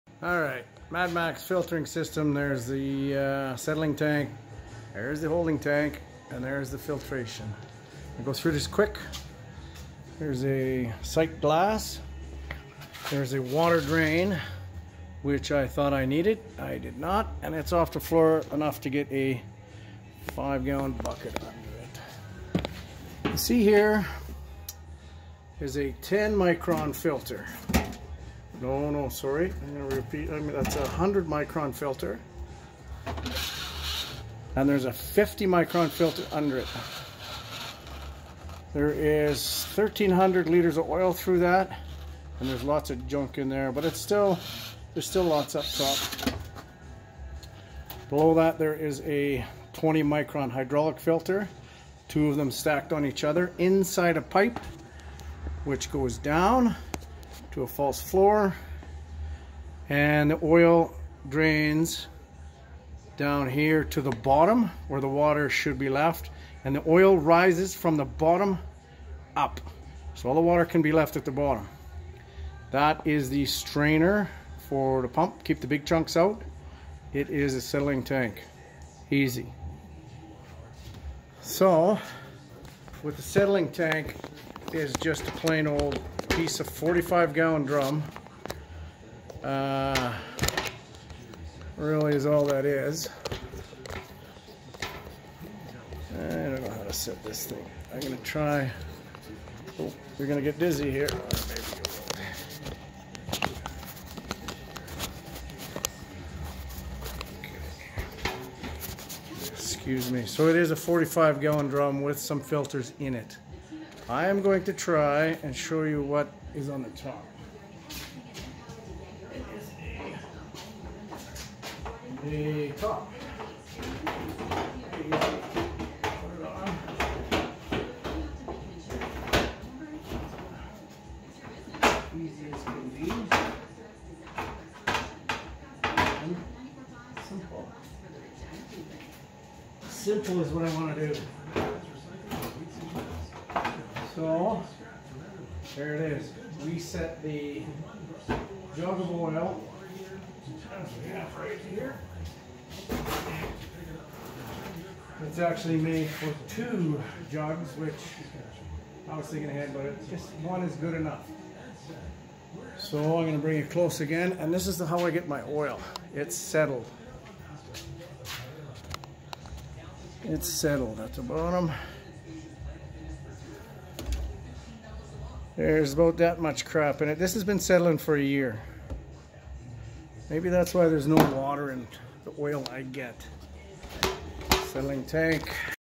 waste vegetable oil, filtration, settling tank, 1of 3, used oil filtration ( MAD MAXX )
this is how I filter mad Maxx's food
first it goes through a 100 micron then 50 micron sock filter   say 4   inches round 15 inches long (bought these on eBay I think)  .. then through a 20 micron (2 stacked) old hydrolic filter  from a big truck.

barrel has a false floor with a big tube to the bottom. theroy here is that the oil fills the barrel from the bottom up.
slow  flow    and settling property's shoud keep the better oil at the top   and water and krud at the bottom.
I should have raised the barrel like 4" instead of 24 " to make putting the jug of oil easier as there is no water in my feed stock.

I don't get the oil  like that. isaid that wrong.
I let it sit for a year

I let it sit for a long time(about a year) which does most of the filtering (crap/water/French frie pieces  settles to the bottom of the jug)